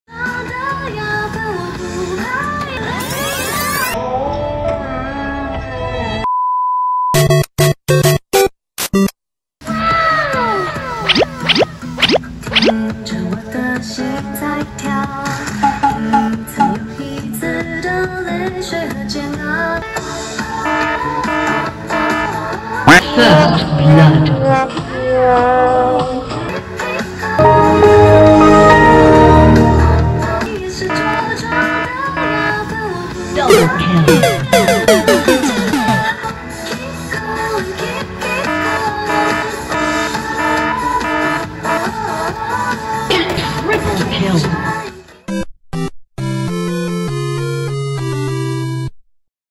驕仔舞連機器人也瘋狂，尬舞到軟腳！！！
一開始帥到我的心在跳，最後我忍不住大笑。

看完影片，笑到併軌，連機器人都會軟腳，我這阿伯跳錯邊跟落拍也是正常的啦…

全中運不只選手動，驕仔舞更是全民運動，大家一起跳起來！

謝謝屏東縣青少年中心 提供影片